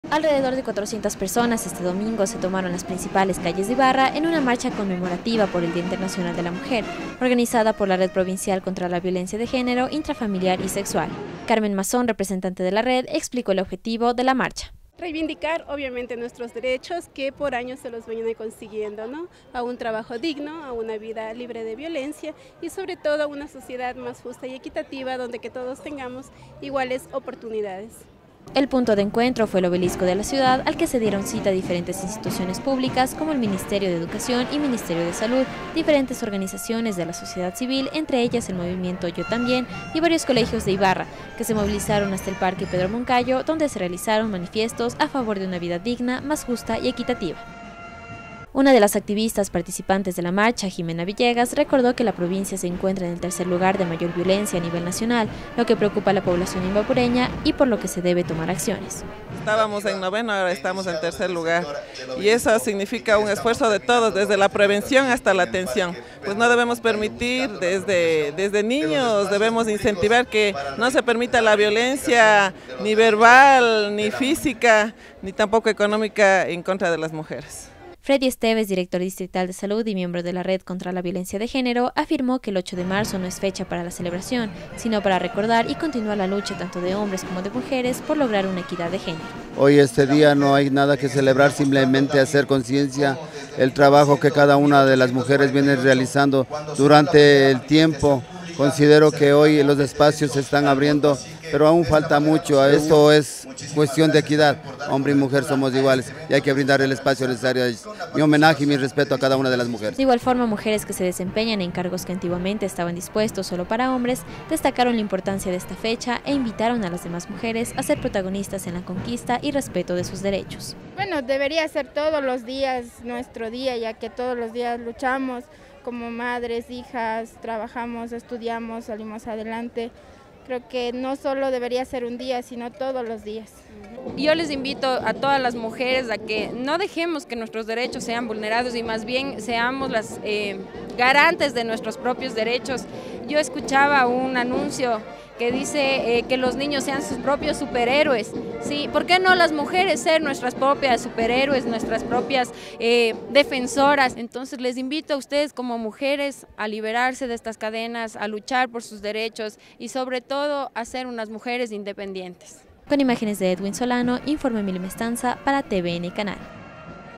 Con una marcha se conmemoró el día interncional de la Mujer en Ibarra
Instituciones públicas, organizaciones sociales y establecimientos educativos participaron de una marcha por las calles de Ibarra en conmemoración  del día internacional de la mujer